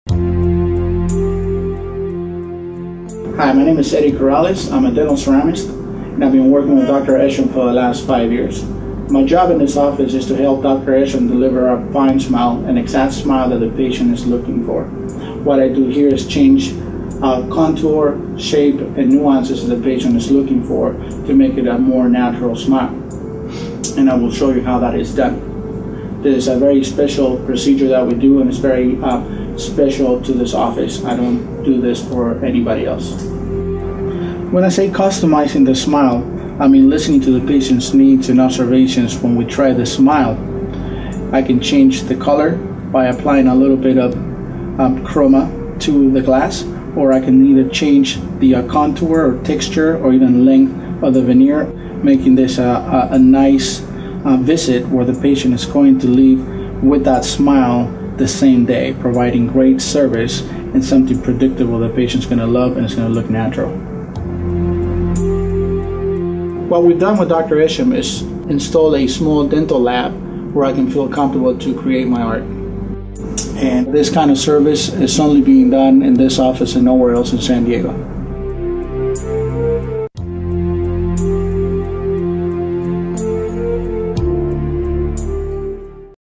Eshom DDS: Dental ceramics with Eddie Corrales
San Diego Cosmetic Dentist works with top dental cosmetic dental ceramist to bring excellence in smile makeover design.  With custom in office shading, ceramist Eddie Corrales has a partial laboratory at Dr. Eshom's dental practice so he can make any color corrections to the porcelain veneers he has hand crafted.  The advantages of custom porcelain veneer shading, means that patients don't have to wait another week for their porcelain veneers to be completed., sandiego-inivsalign.com